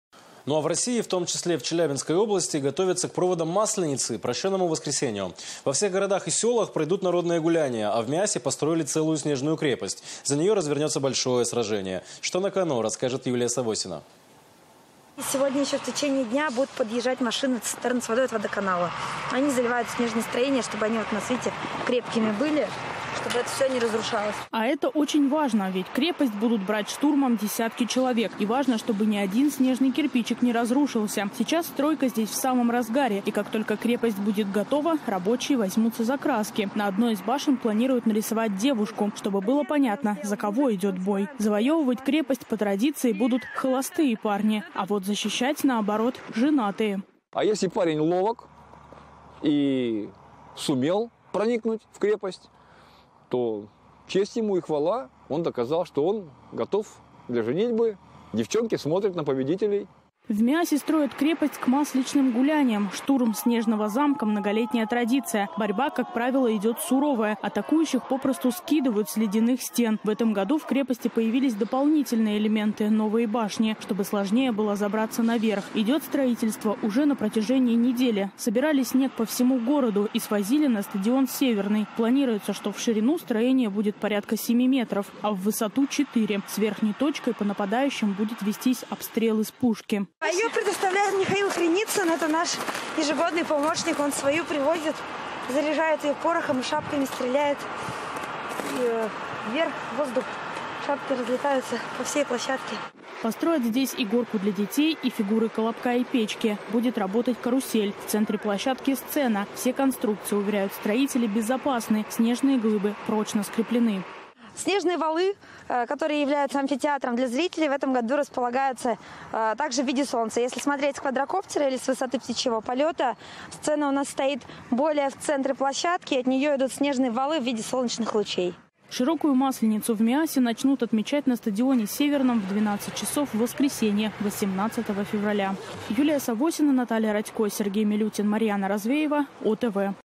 Для народных гуляний строят снежную крепость
Ну, а в России, в том числе и в Челябинской области, готовятся к проводам Масленицы, прощенному воскресенью. Во всех городах и селах пройдут народные гуляния. В Миассе построили снежную крепость. За нее развернется большое сражение. Что на кону, Юлия Савосина расскажет.

А это очень важно. Ведь крепость будут брать штурмом десятки человек. И важно, чтобы ни один снежный кирпичик не разрушился. Сейчас стройка здесь в самом разгаре. И как только крепость будет готова — рабочие возьмутся за краски. На одной из башен планируют нарисовать девушку. Чтобы было понятно, за кого идёт бой. Завоёвывать крепость по традиции будут холостые парни. А вот защищать, наоборот, женатые.